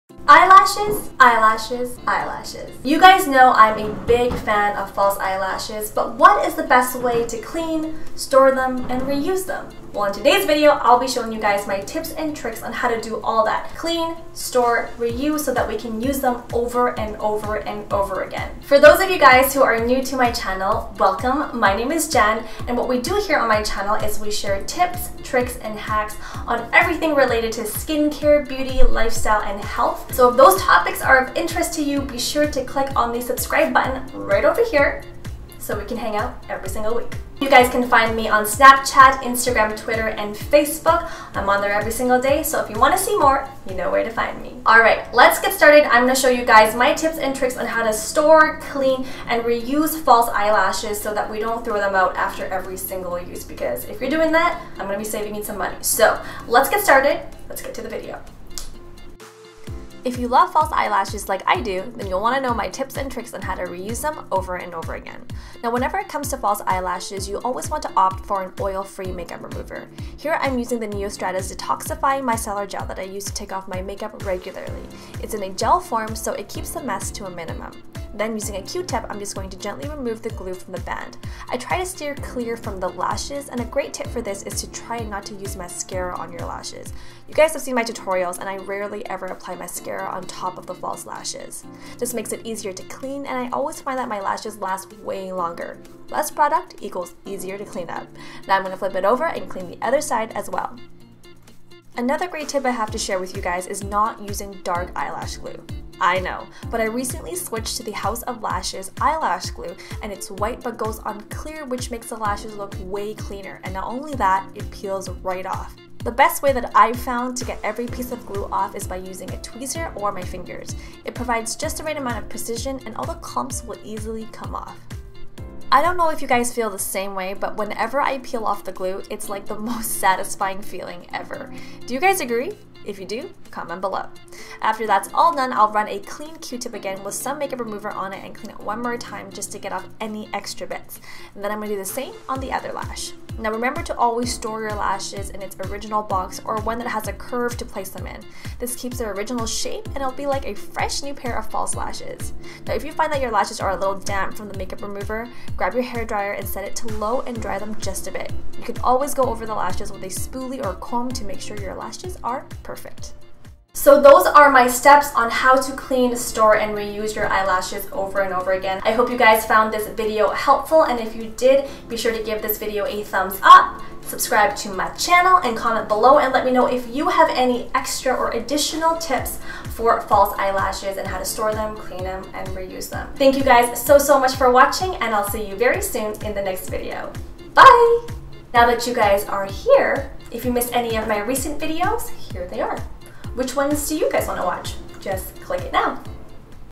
HOW TO: Clean, Store and Reuse False Eyelashes | chiutips
MAKE YOUR FALSE EYELASHES LAST 20X! Here is my step-by-step tutorial on HOW TO: Clean, Store & Reuse False Eyelashes so that you can wear them over and over again. They’ll look brand new and you’ll save money by reusing them!

LET’S HANG OUT EVERYDAY!

Camera & Lighting

and Natural Lighting!